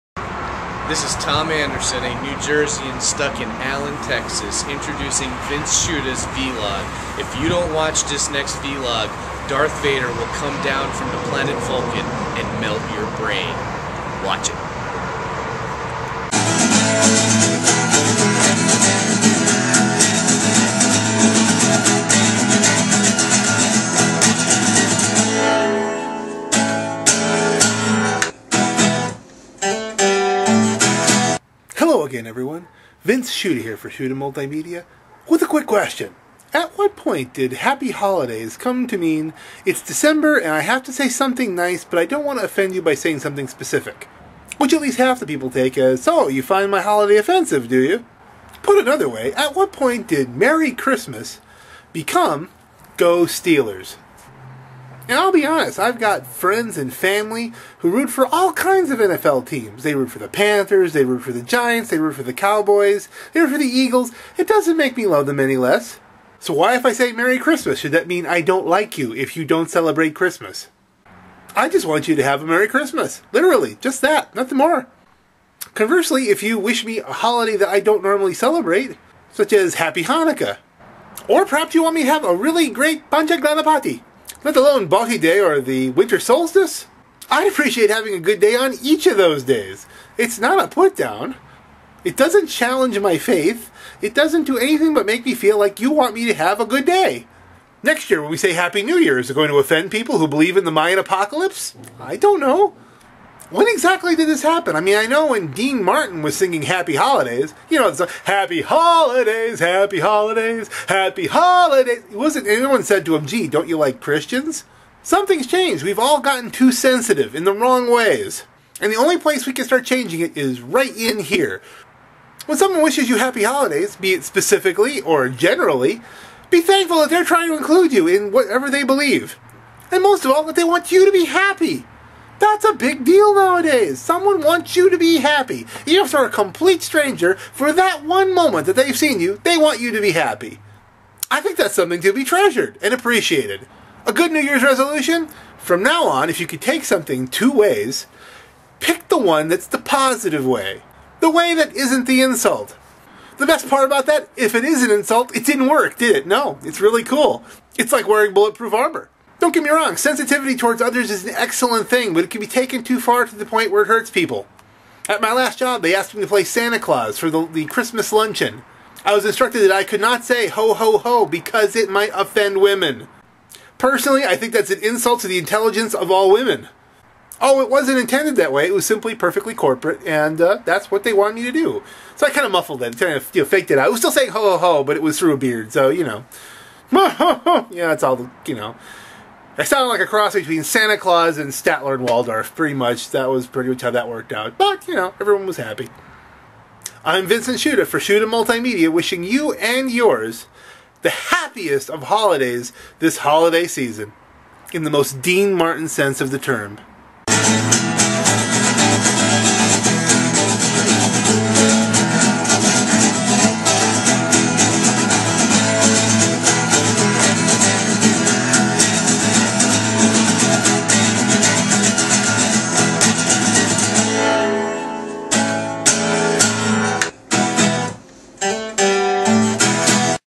Happy Holidays: Episode 77 of the Shuta Multimedia Vlog
On behalf of everyone at Shuta Multimedia, I'd like to wish everyone the Happiest of Holidays, whatever holiday that may be. And I also wish that you take that in the true spirit in which it was intended. Don't know where I'm going with that? Check out the video. :-)

Special apologies to Andy Williams, who is not Dean Martin as I thought. Check out one of my favorite holiday classics

And as an extra special bonus, check out this video by our friends over at Community Film Project: